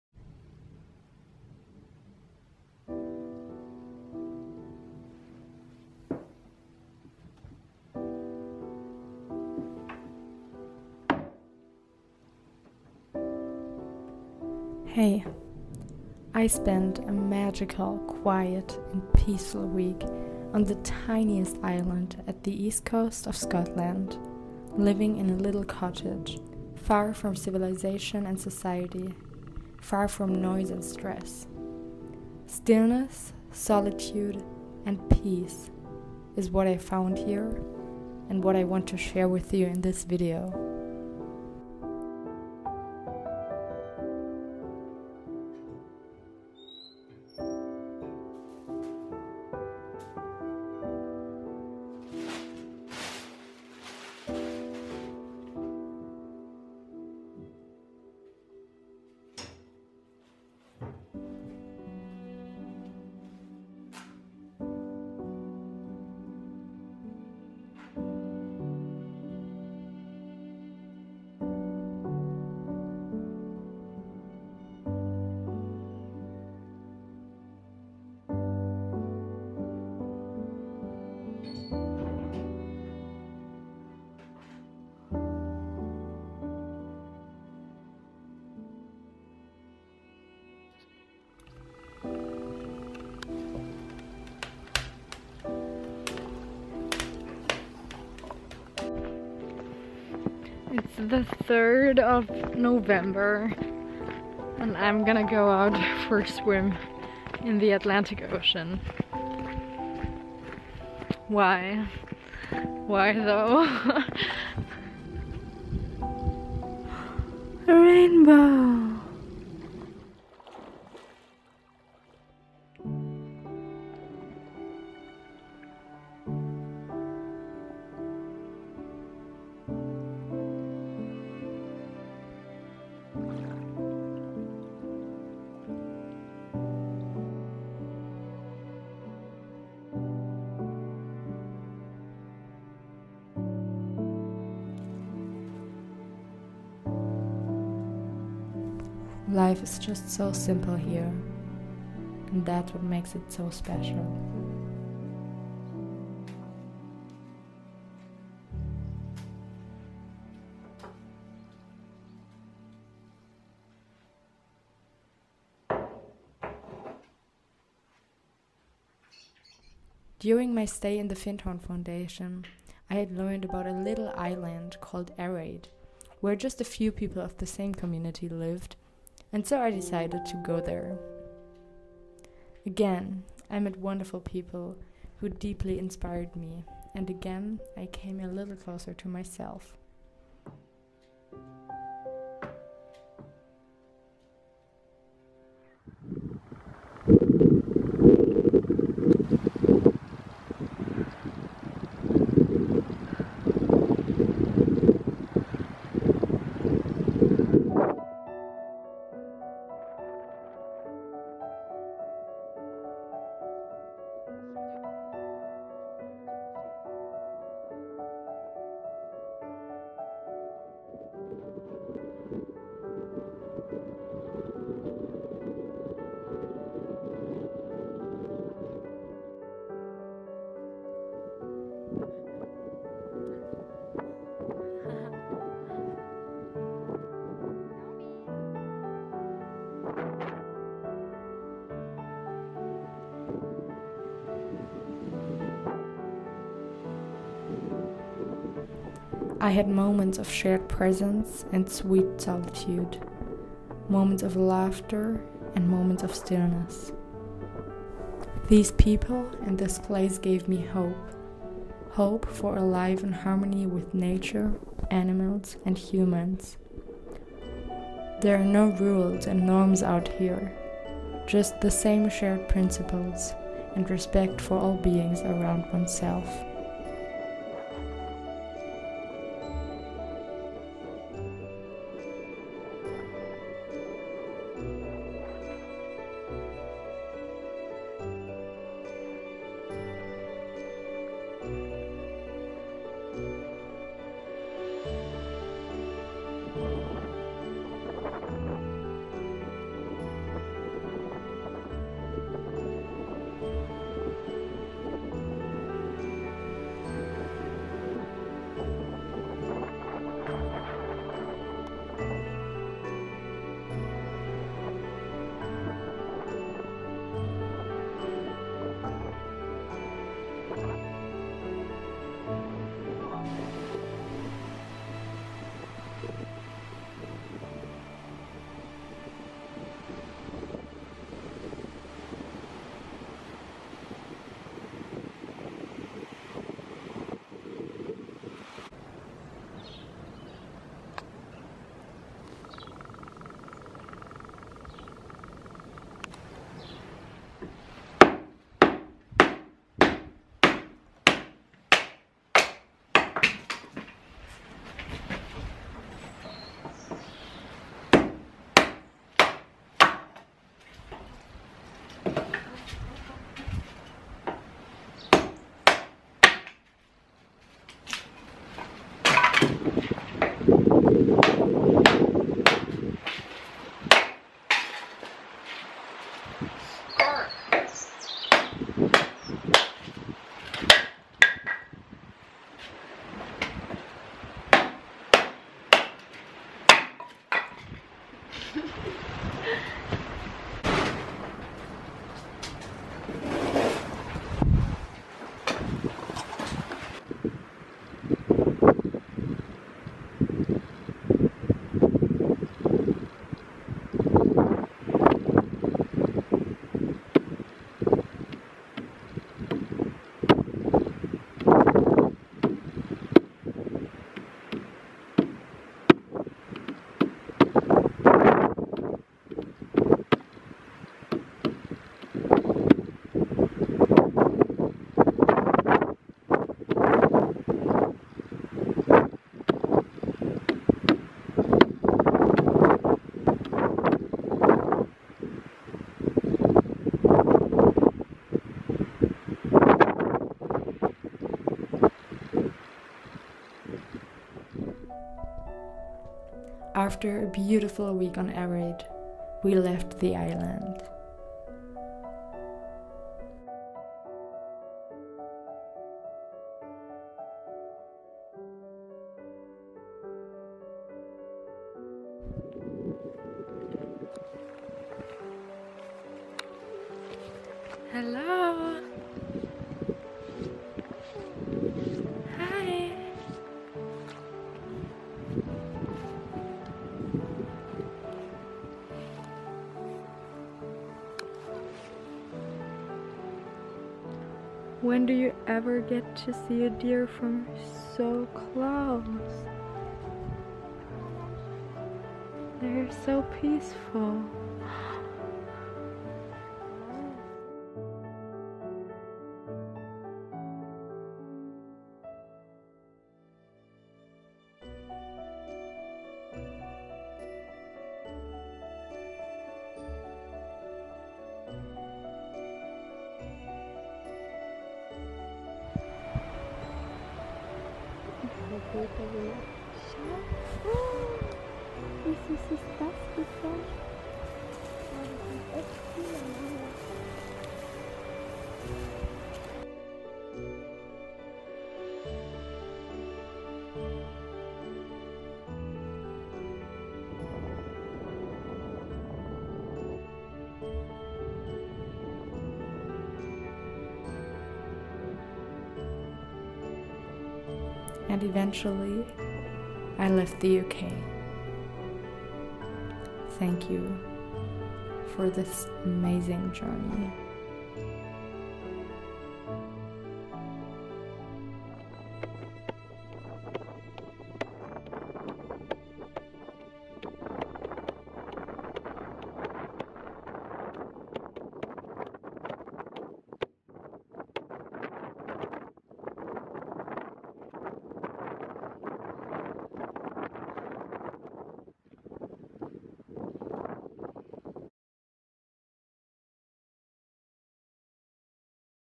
Erraid Findhorn Foundation Community | Living on a tiny island in a tiny cottage
even though this video comes incredibly late, I hope I can still give you a sense of my time on Erraid with the Findhorn Foundation Community. (I was there in November ‘23)
“Stillness, solitude and peace, is what I found here and what I want to share with you.”
I had the most beautiful, peaceful and quiet week of my life out there on this stunningly beautiful island at the east coast of Scotland.
My road trip through the Scottish highlands ended three months ago and I am just about to start my new journey… I’ll keep you posted here on my YouTube channel.

Sending you my love out there

Stella 💕✨